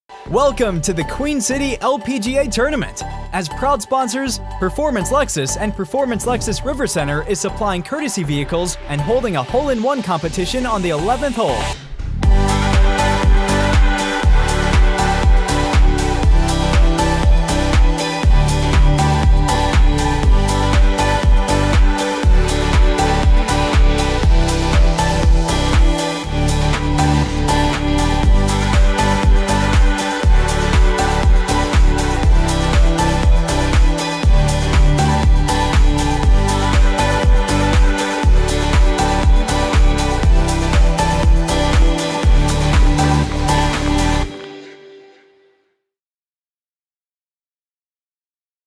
Proud Sponsors of the Queen City LPGA Tournament | Performance Lexus
Congratulations to everyone who participated in the 2022 Queen City LPGA Tournament!